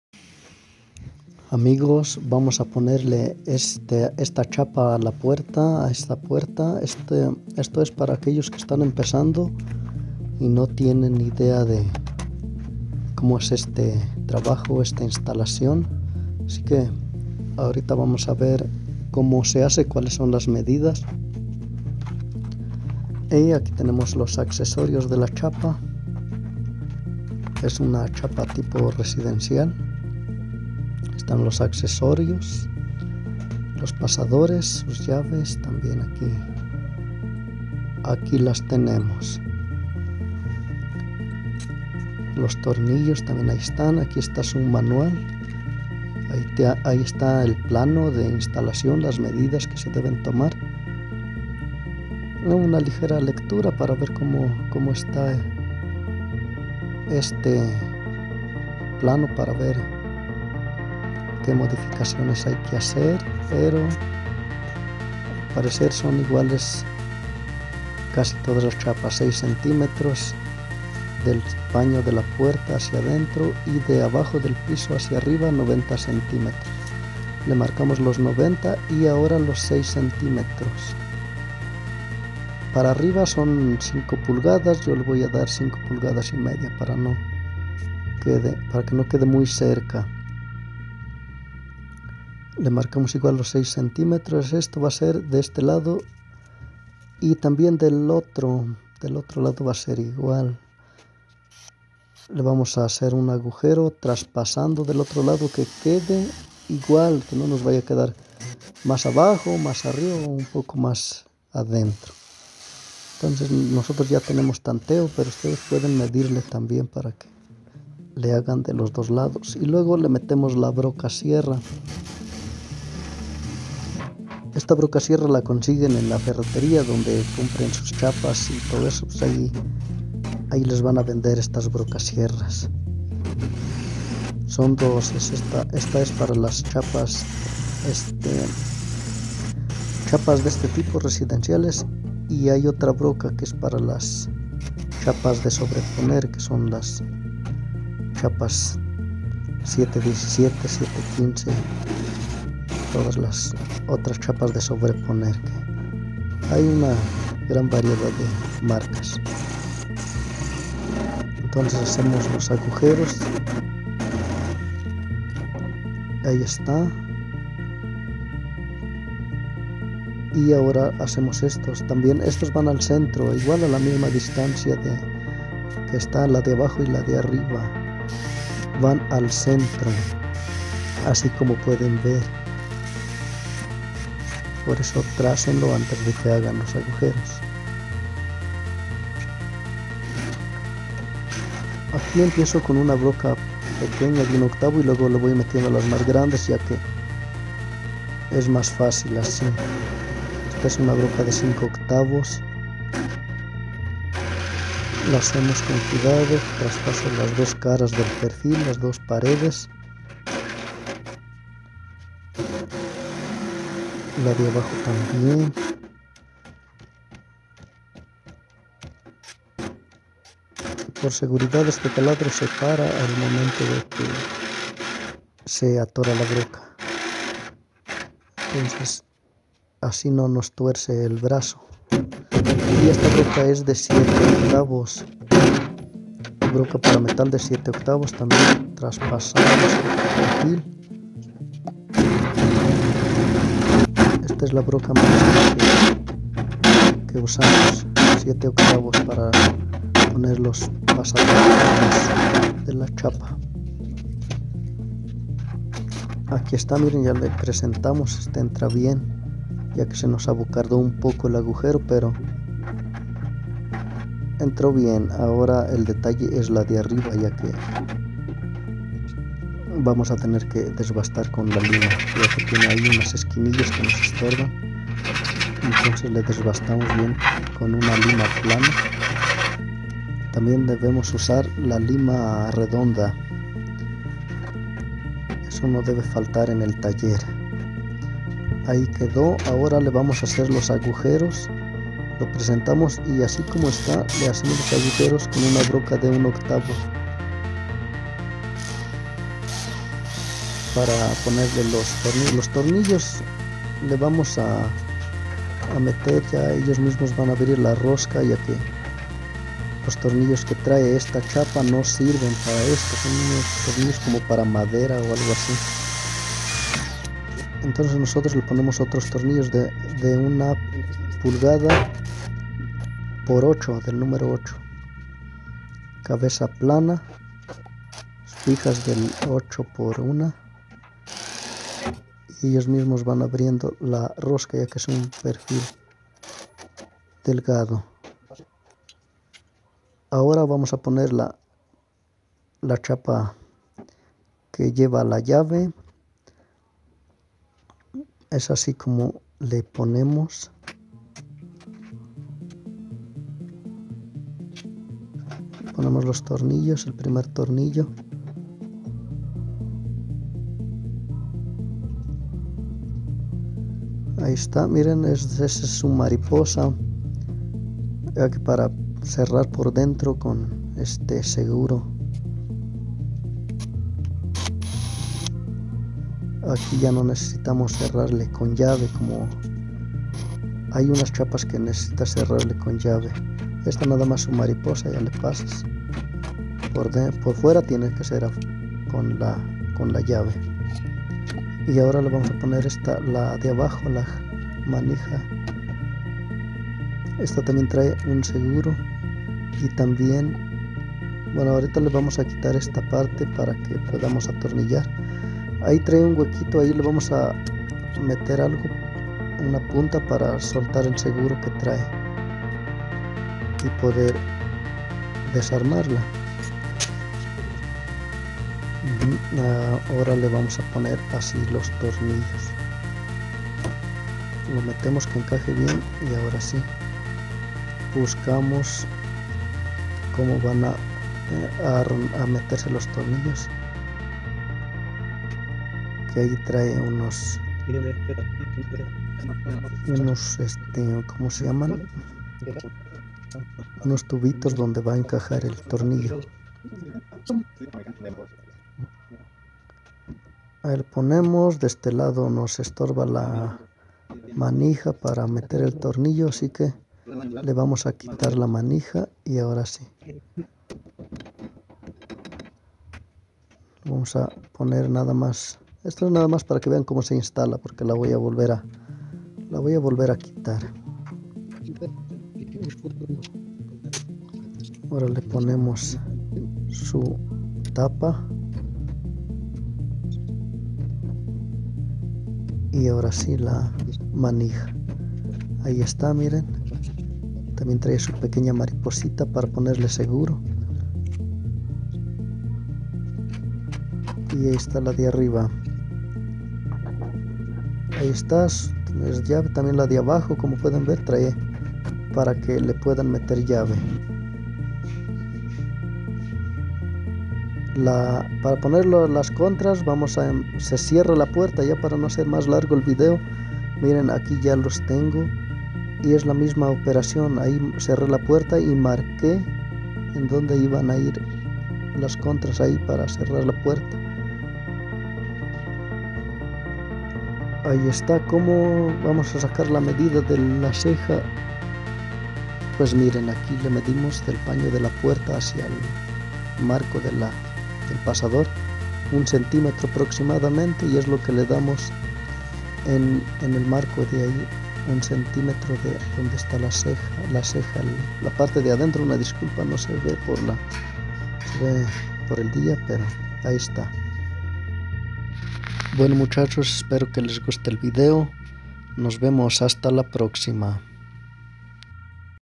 COMO INSTALAR UNA CHAPA O CERRADURA RESIDENCIAL EN PUERTA DE HERRERÍA
Los mejores diseños de puertas, ventanas
Portones,barandales y protectores todo
En herrería